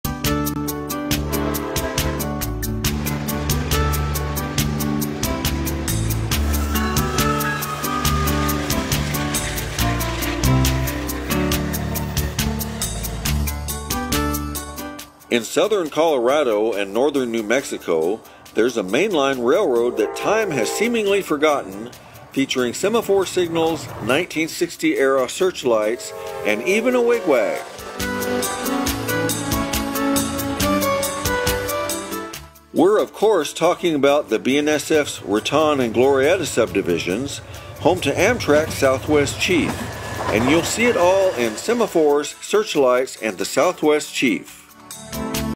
Have you seen this film? Comment: yes or no
This is our program covering the BNSF's Raton and Glorieta Subdivisions and the Southwest Chief.  See the line from La Junta, Colorado to Albuquerque, New Mexico. See all three stands of semaphores before the 2020 signal replacements.

There's more to this line than the signals, there's great scenery too!  See trains 3 & 4 on the Great Plains, Raton Pass, Glorieta Pass, Shoemaker Canyon, Apache Canyon and more!